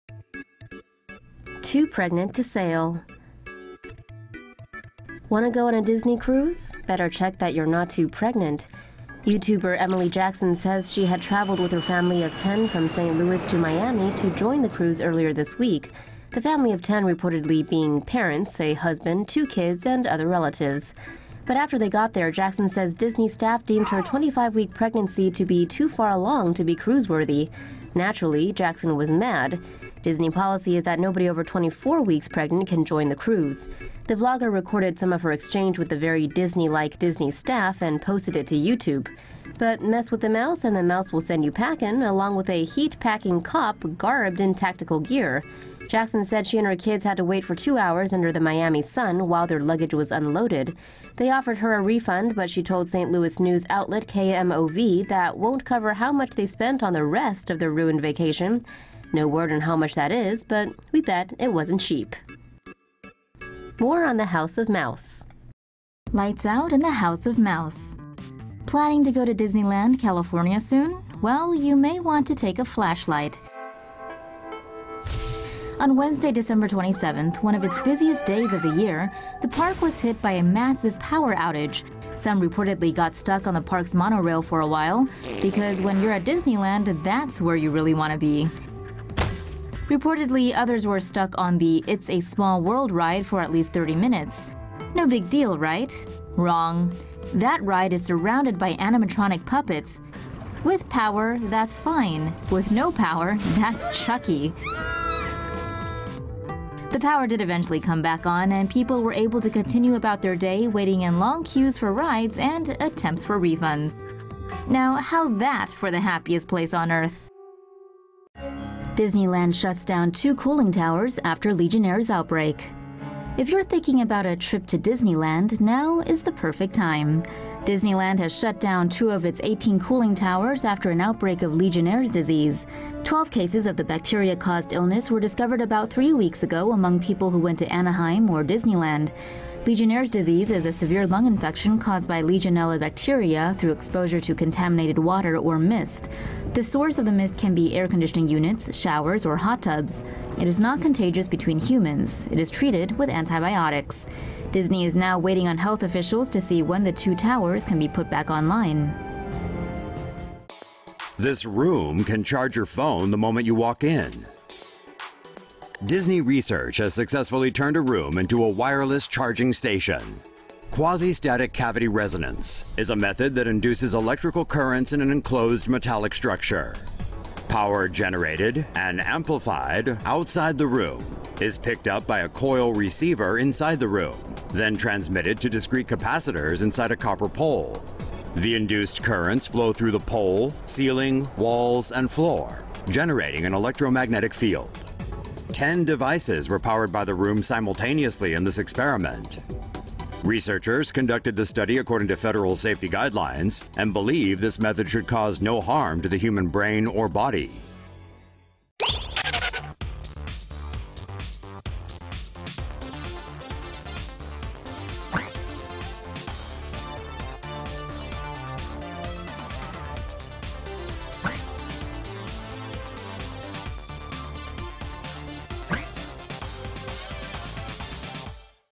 Cop escorts 'too pregnant' mom and family from Disney cruise - TomoNews
MIAMI — YouTuber Emily Jackson says she had traveled with her family of 10 from St Louis to Miami to join the cruise earlier this week.

The family of 10 reportedly being parents, a husband, two kids and other relatives.

After they got there, Jackson says Disney staff deemed her 25-week pregnancy to be too far along to be cruise-worthy. Naturally, Jackson was mad.

Disney policy is that nobody over 24 weeks pregnant can join the cruise.

The vlogger recorded some of her exchange with the very Disney-like Disney staff and posted it to YouTube. But mess with the mouse, and the mouse will send you packin' along with a heat packing cop garbed in tactical gear.

Jackson said she and her kids had to wait for 2 hours under the Miami sun while their luggage was unloaded. They offered her a refund but she told St Louis news outlet KMOV that won't cover how much they spent on the rest of their ruined vacation.